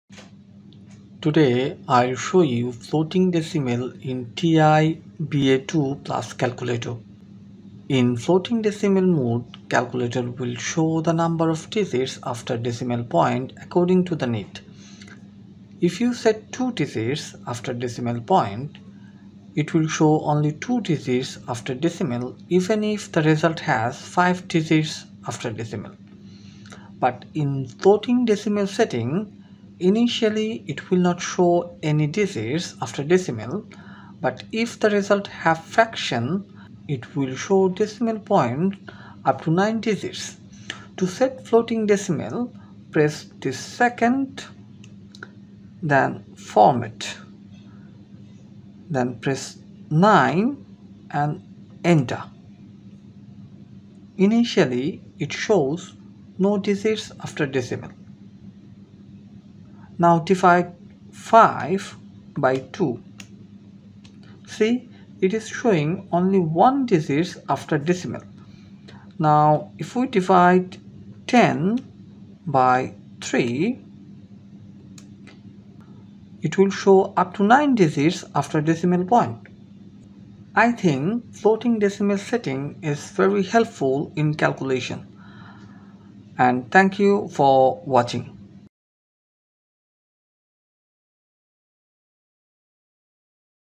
Auto decimal placement (Floating Decimal) in TI BA Plus Financial Calculator
Floating Decimal in TI BA Plus Financial Calculator :
Floating decimal make it easy to understand results. In floating decimal there will be no fixed decimal places, the calculator will show the result according to the need. This is a system of decimal point placement in an  calculator where decimal point is free to move automatically across the display in order to allow the maximum number of decimal places in the readout.

0:00- floating decimal (decimal according to need)